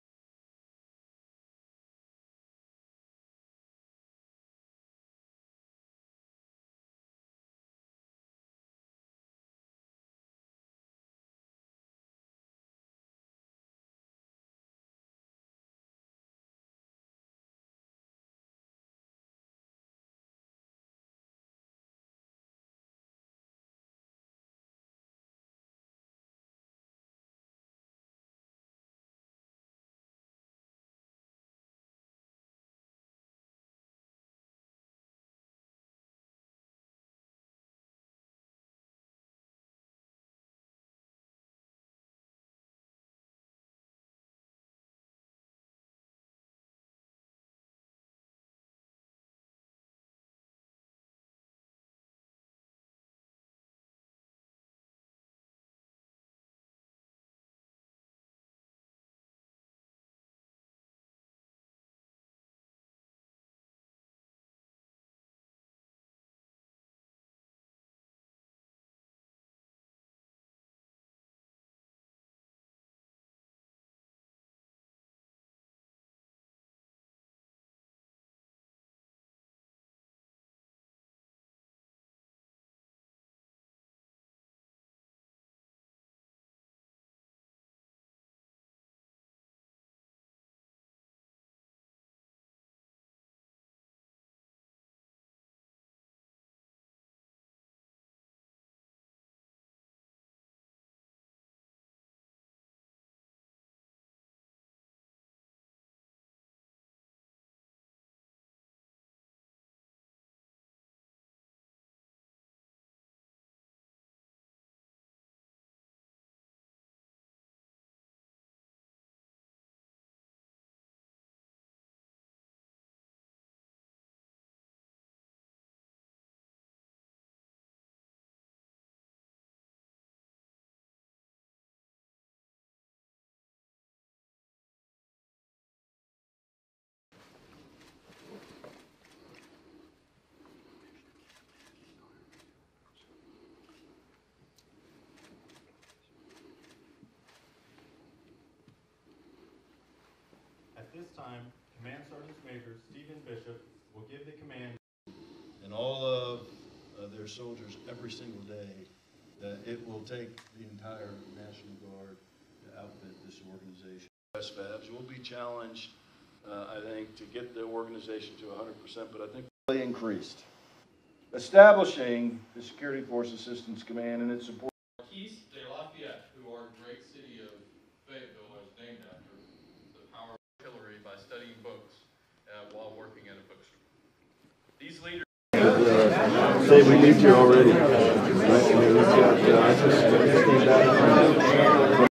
The Army Recognizes the 54th SFAB
Credit: Staff Sgt. Cesar Cordero | Date Taken: 03/05/2020
The Department of the Army recognizes the 54th Security Forces Assistance Brigade in an official ceremony at Ft. Bragg